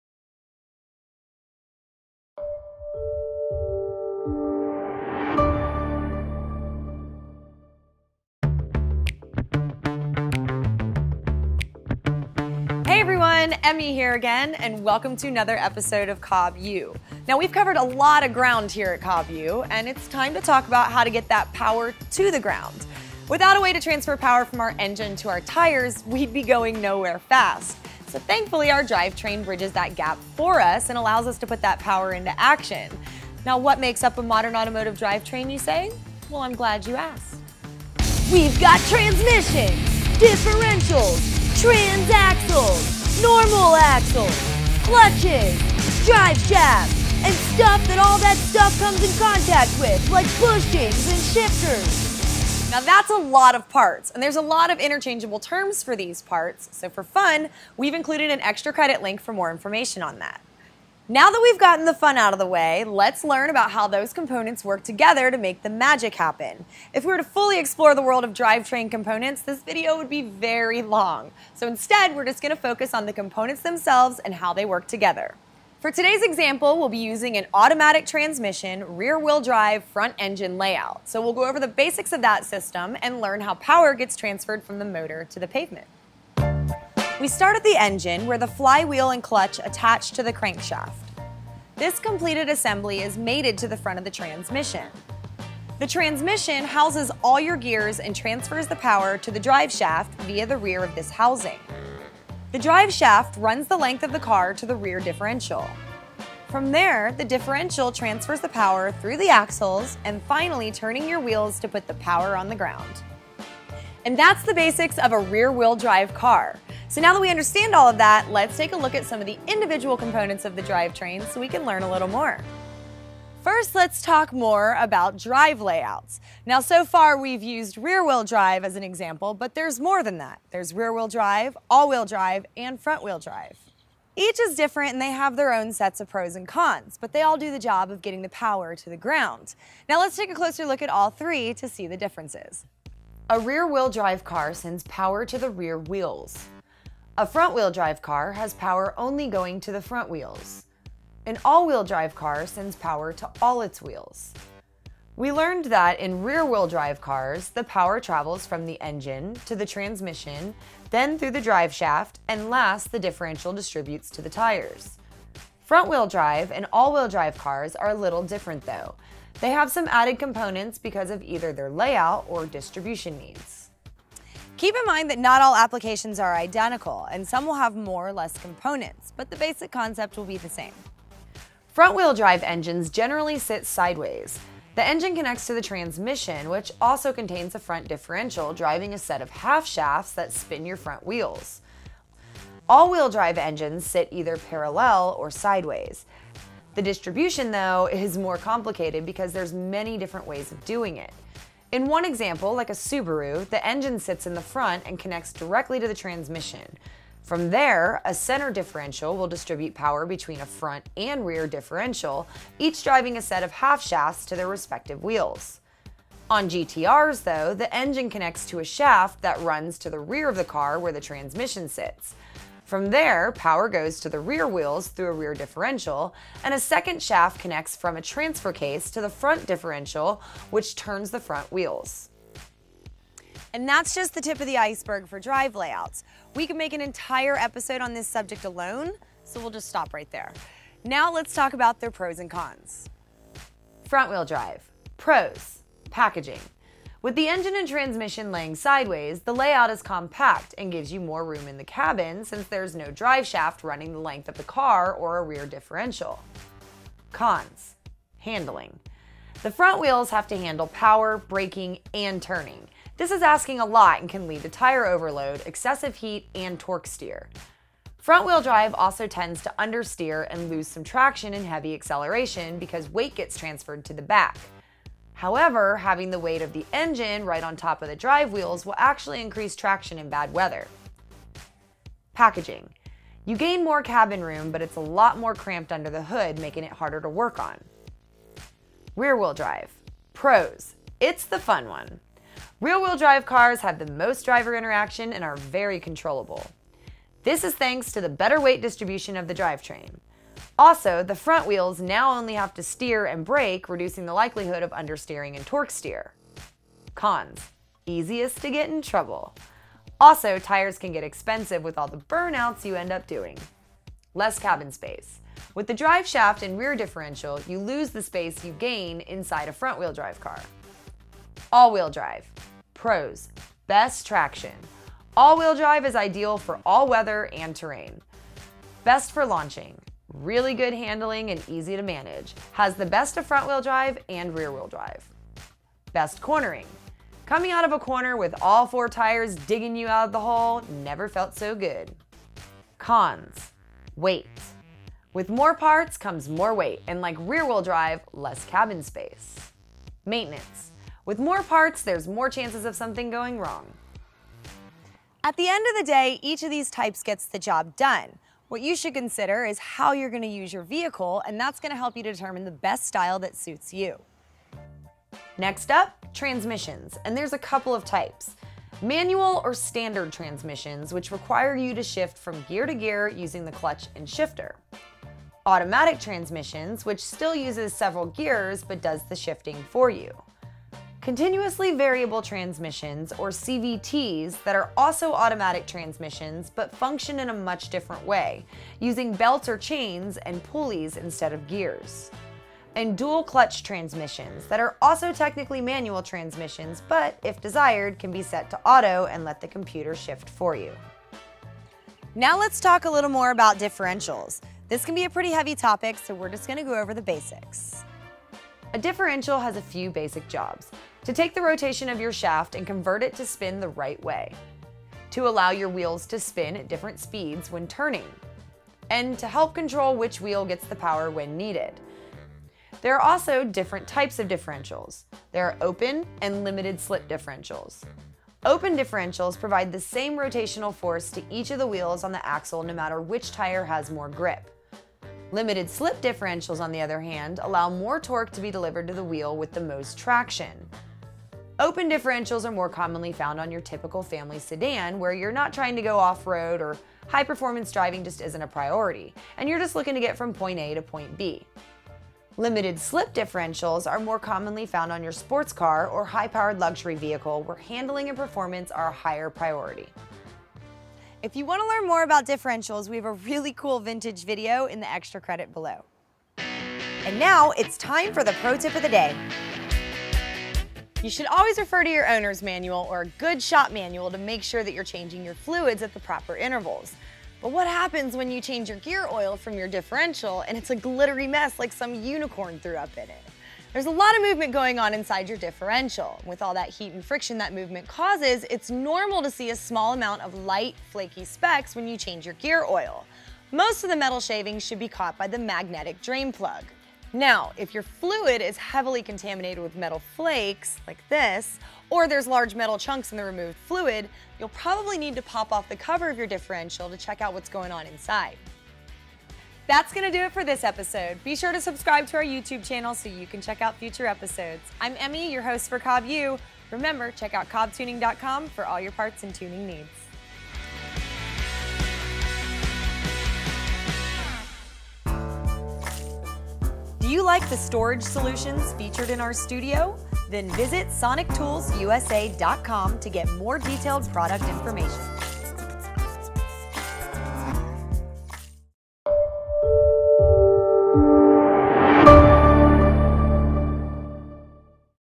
COBB Tuning - COBB University Episode #12 - Drivetrain
FWD? RWD? AWD?  What does it all mean?  Is one better than the other?  In this episode of COBB U we'll cover the main components of drivetrains and explain how they all work together to distribute power from your vehicle's engine out to the wheels.

Like the tools and storage solutions in our studio?